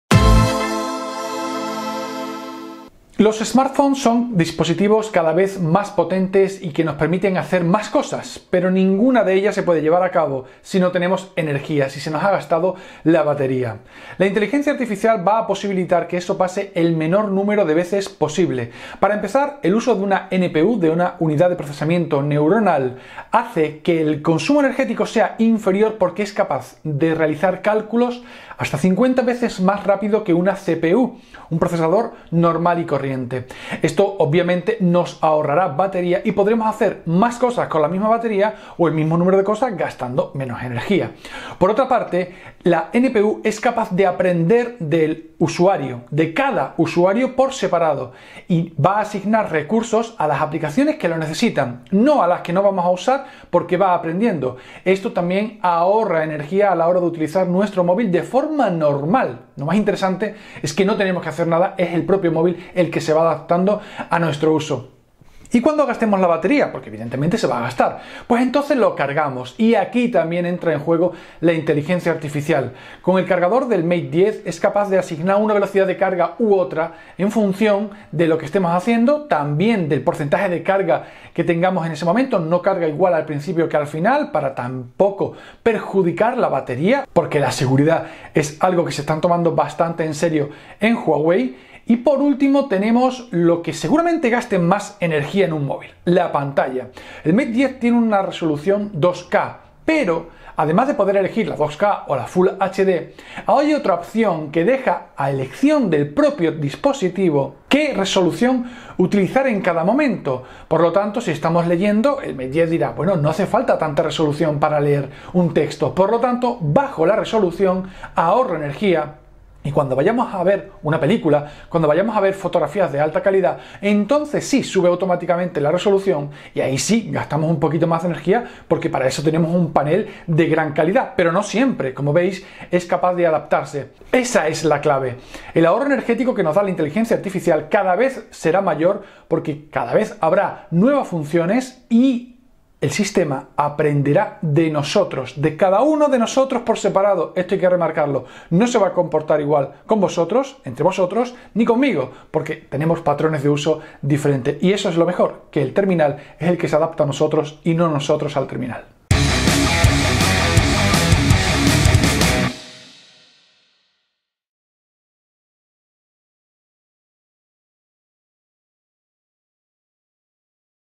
Cómo ayuda a la batería la inteligencia artificial de los smartphones
Música